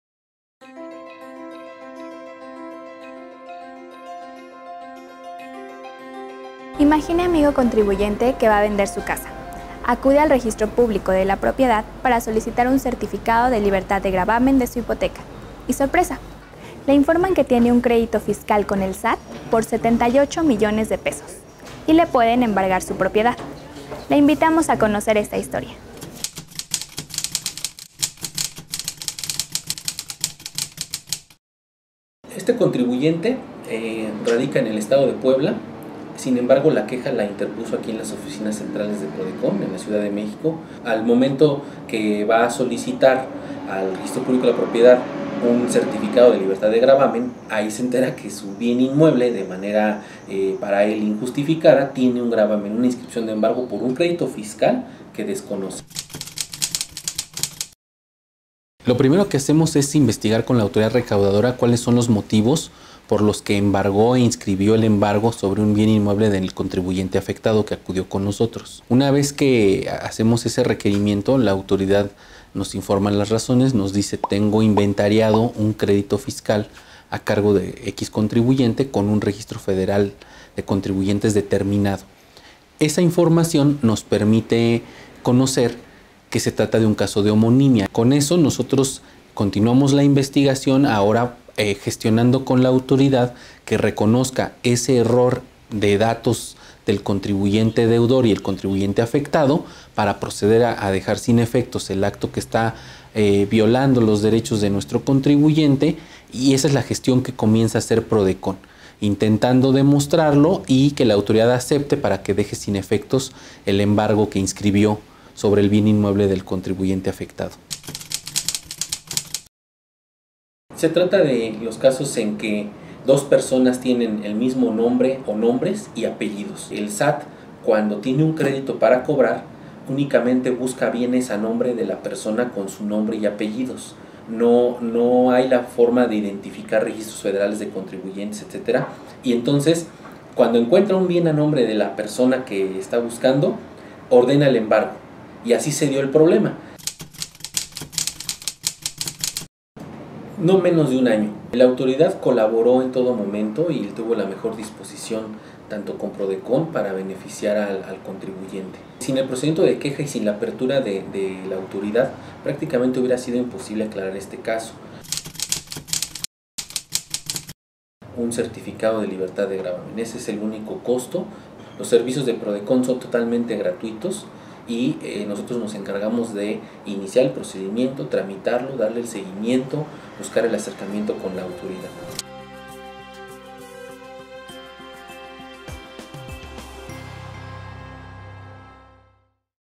Prodecon Informa: Se logra cancelación de inscripción de embargo a bien inmueble de contribuyente
El Subprocurador de Protección de los Derechos de los Contribuyentes, Andrés López Lara, explica que al solicitar un certificado de libertad de gravamen ante el Registro Público de la Propiedad y de Comercio, un contribuyente se percató de que el inmueble de su propiedad tenía una anotación de embargo relacionada con un crédito fiscal que manifestó desconocer, por lo que interpuso una Queja ante PRODECON.

Derivado de las gestiones realizadas por esta Procuraduría se conoció que una autoridad estatal había solicitado dicha inscripción a fin de realizar el cobro coactivo de un crédito determinado a un homónimo del quejoso, por lo que se realizaron acciones de investigación con aquélla a fin de evidenciar tal situación y el resultado permitió la cancelación del gravamen ordenado.